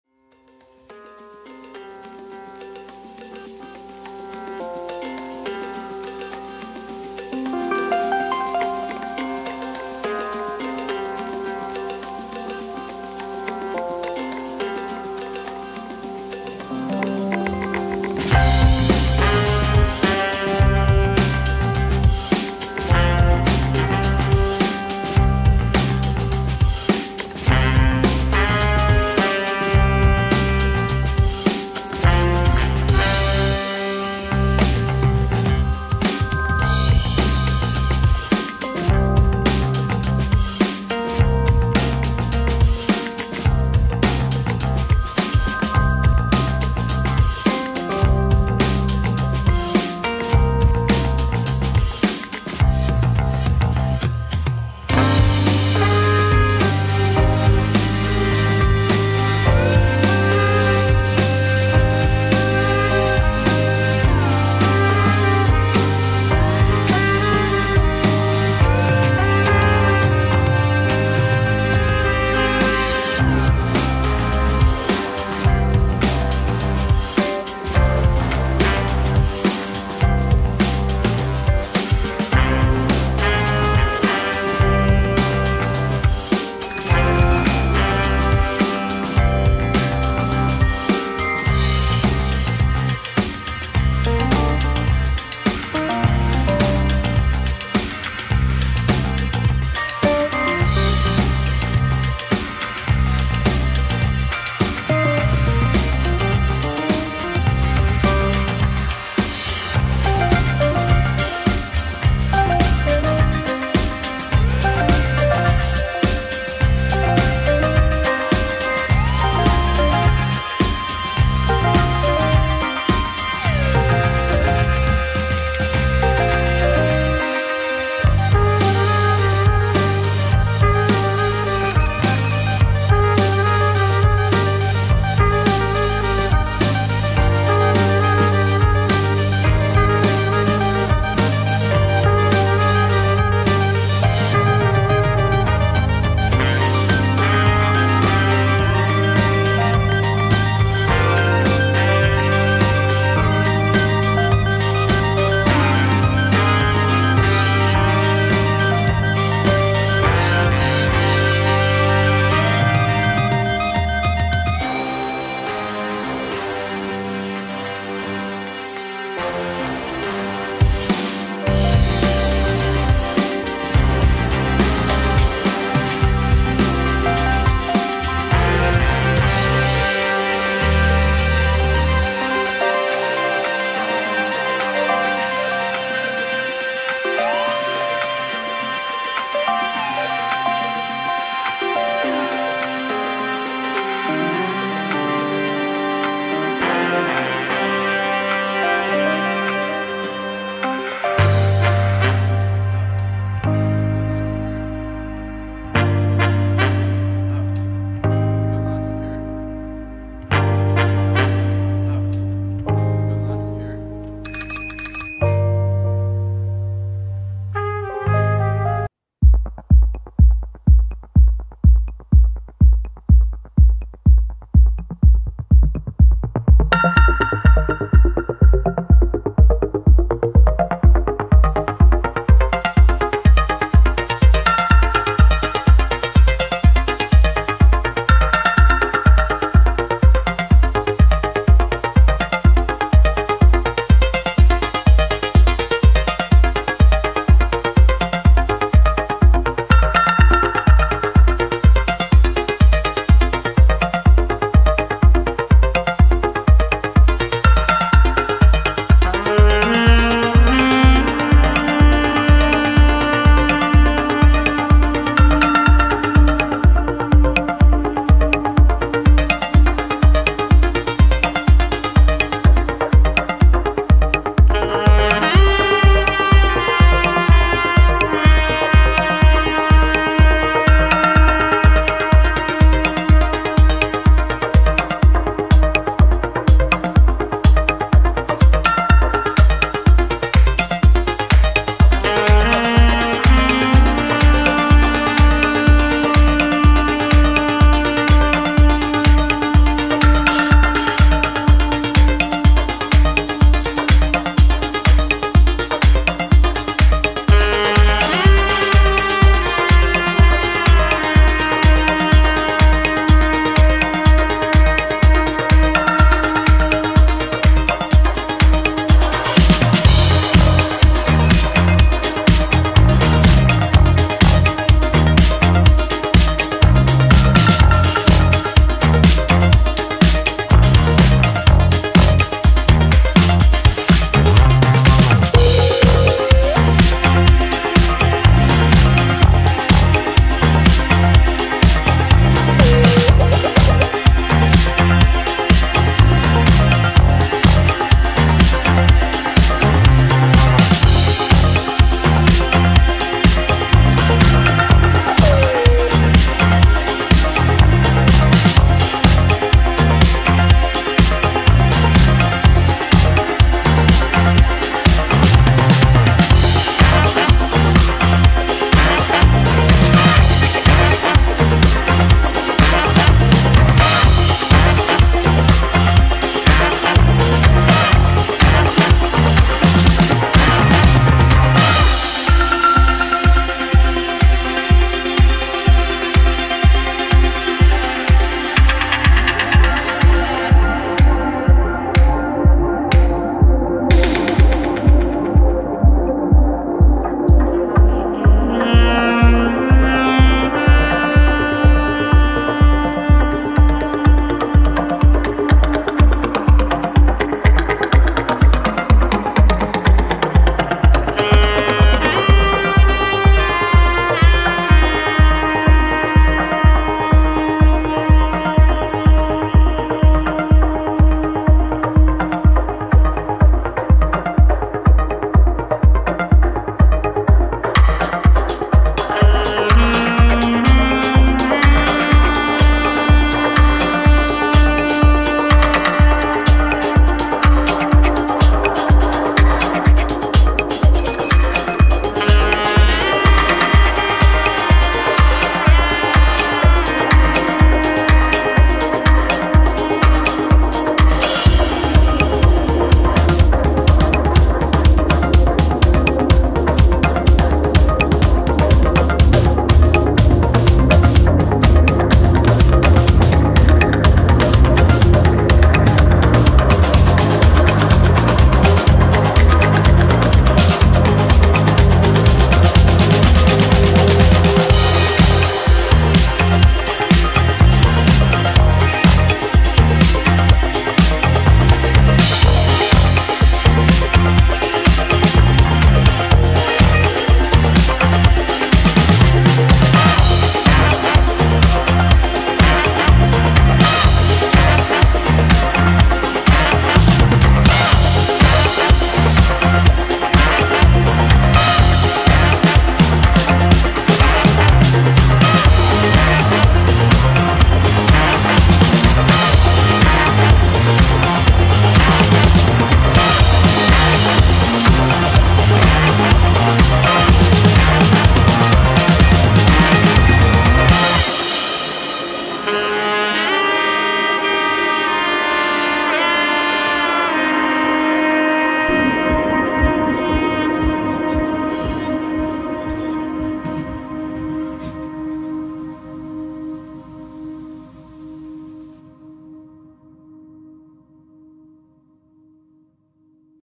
195 Coutcries Powerful Passports in the 2024
World Most Powerful Passports in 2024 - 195 Countries Compared.